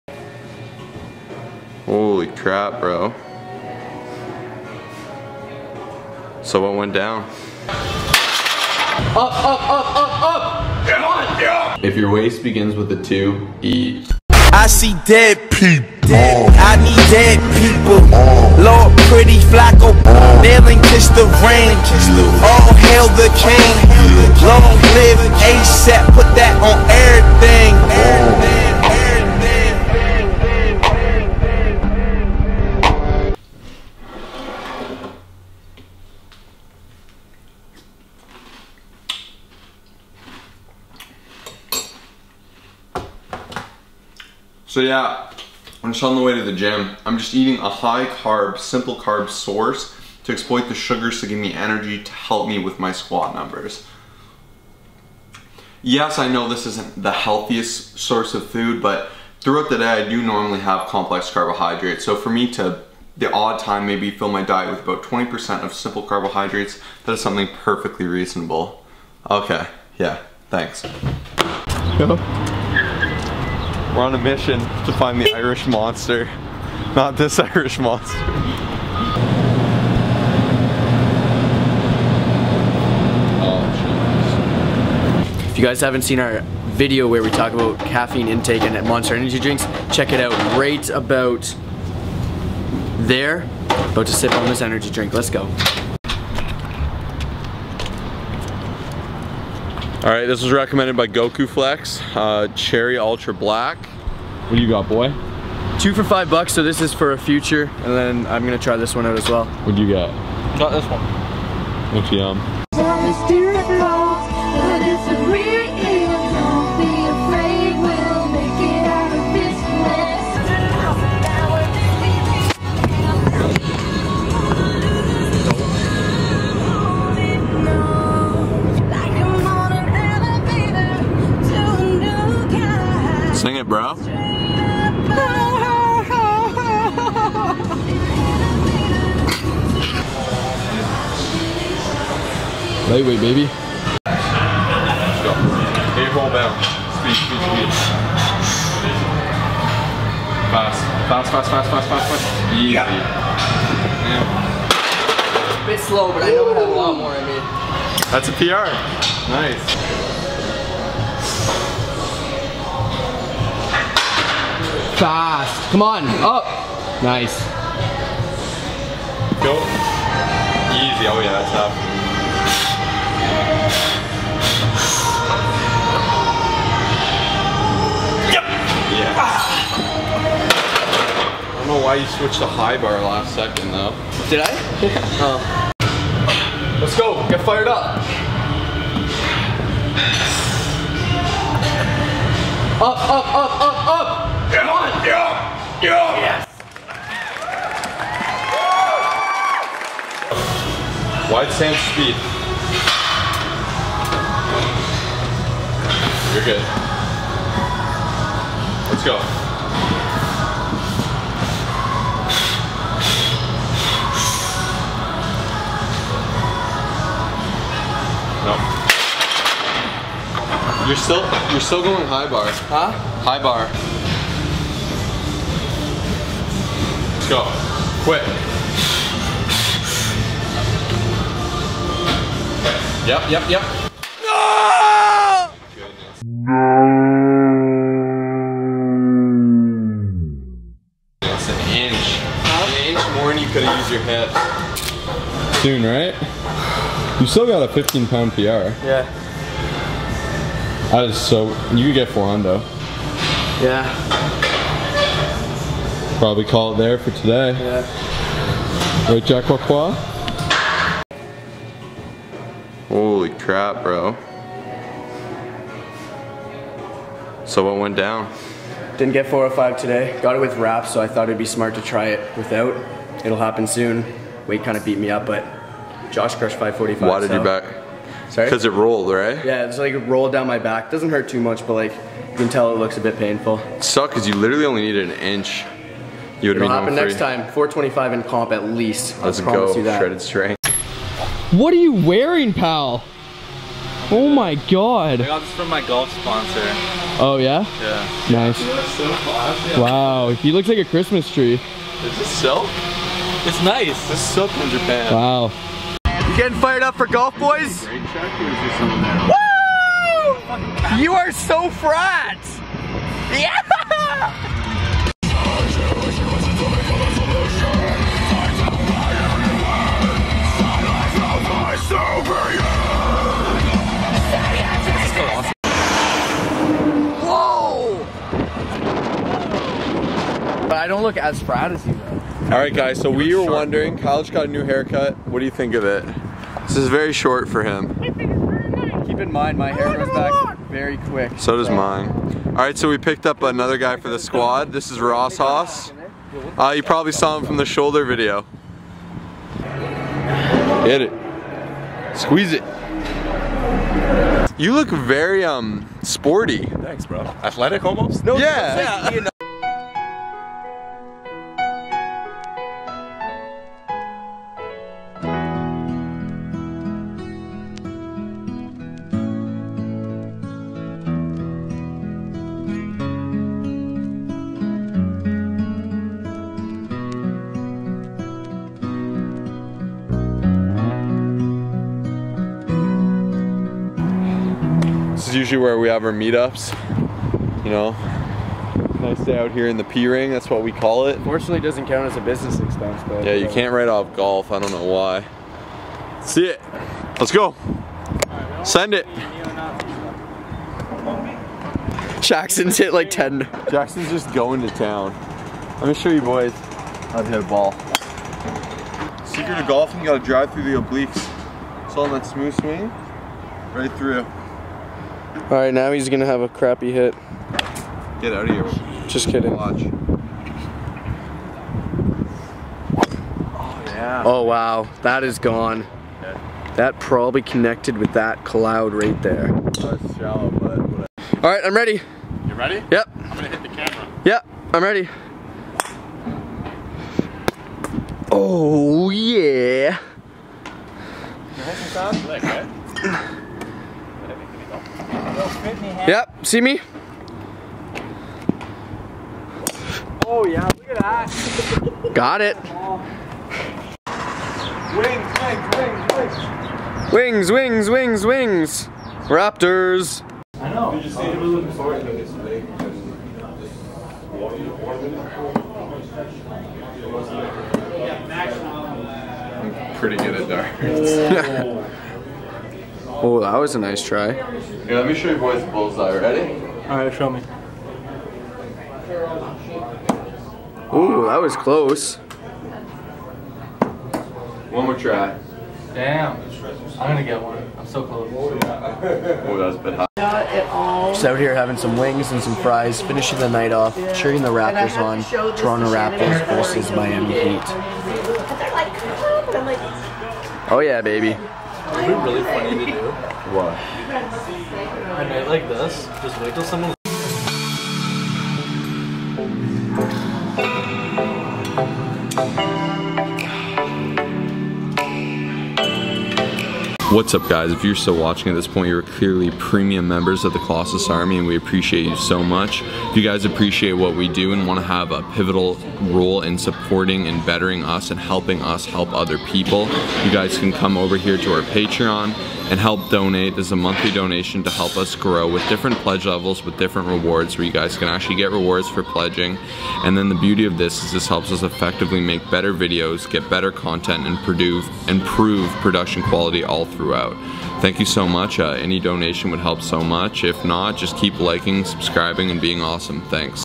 Beat up by the Weight | Shredded Strength Ep. 3
Got my ass kicked by some heavy weight. Also hit some squat pr's and went golfing with some buddies!

BOOK FREE INFO SESSION:
➢ colossusfitness.youcanbook.me

Professional Inquiries and Book/Contact
➢ colossusfitness.youcanbook.me